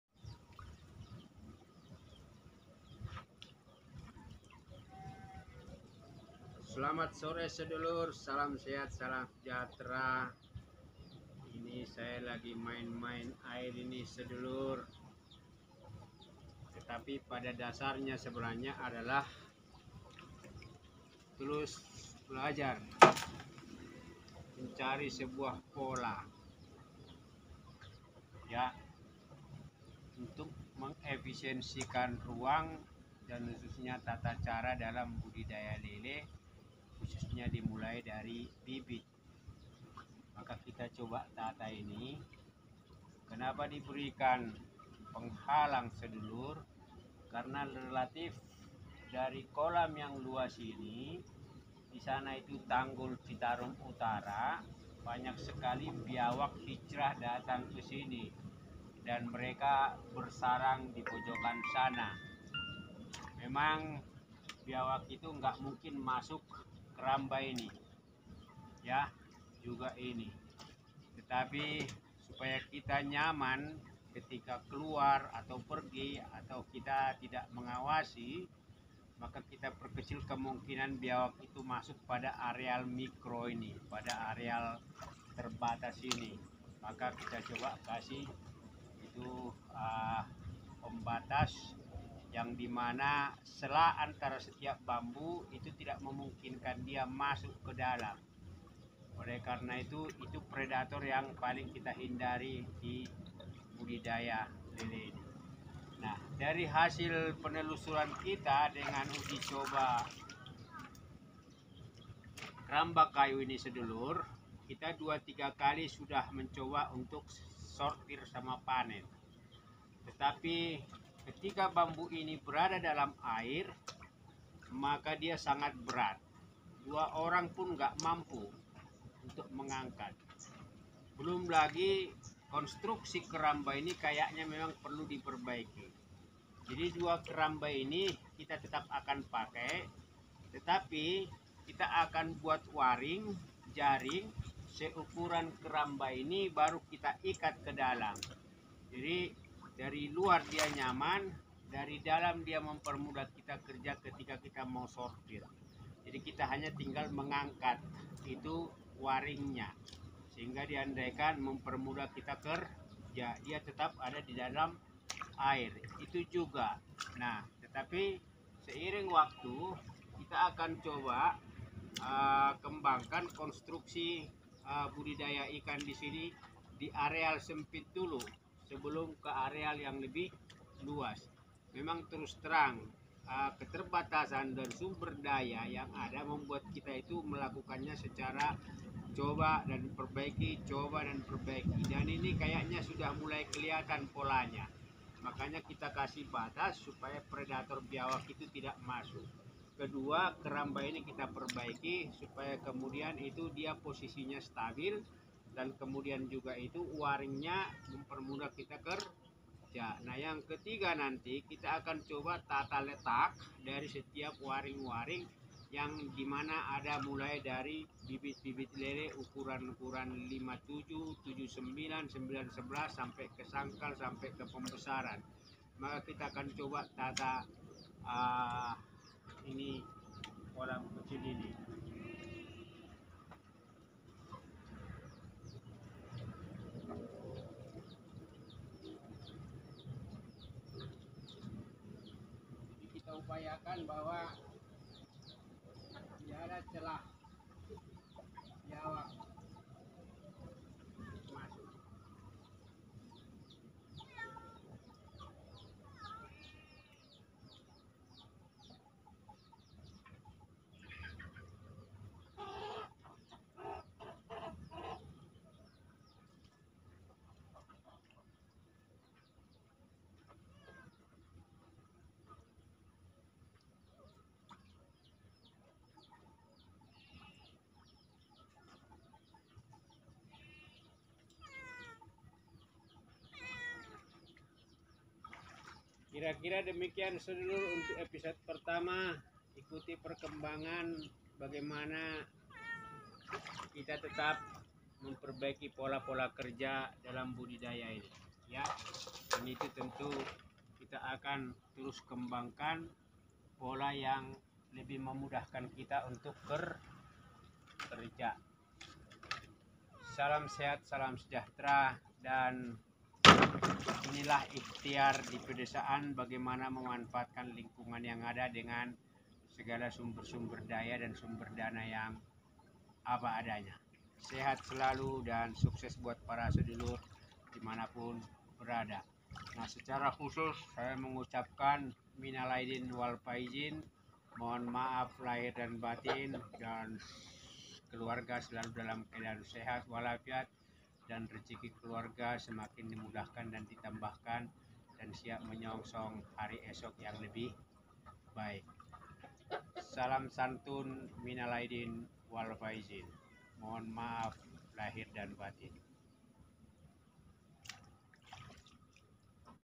Budidaya lele dalam keramba belum berhasil. Redesign.Cari pola efektif
Budidaya lele dalam keramba cukup efektif bila didalam keramba dipasang warung yang mudah diangkat setiap sortir atau panen Cari pola,agar budidaya dimaksimalkan budidayalele,pakan organik